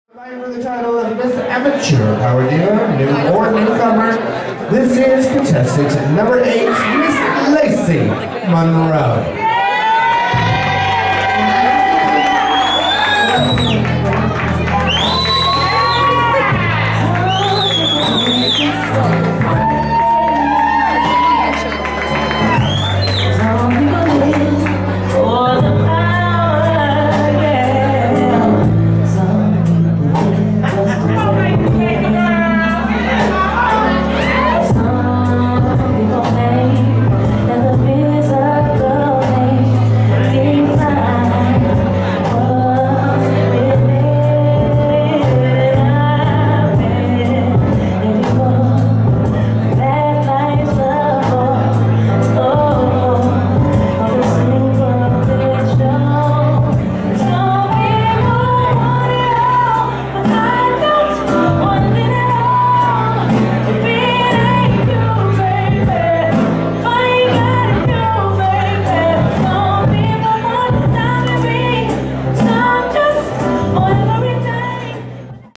Lacey Monroe Gown
Nine contestants compete for the titles of Miss Amateur PowerDiva and PowerDiva Newcomer. Miss Amateur PowerDiva Sarina Le Blonc-Sanchez and PowerDiva Newcomer Armani Evangelista will be handing down their titles. Hosted by Nickki Stevens with performances from Deja Dellataro, Dice Santana, Anna Mossity and Eva Angelica. Filmed at Rumors Nightclub and filmed by Daydream Studios.